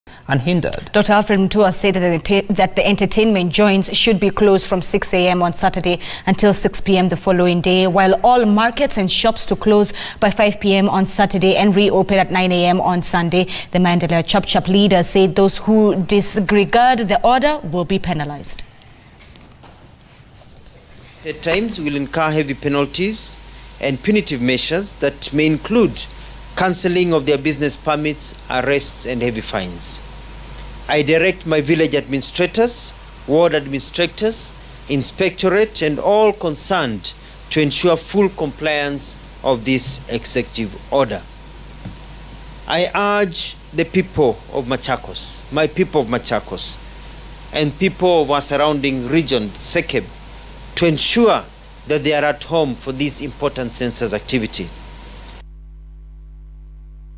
Machakos governor orders closure of bars and entertainment joints on census weekend
Machakos governor Dr Alfred Mutua has ordered the closure of all bars and entertainment joints next weekend to allow the census exercise to proceed unhindered.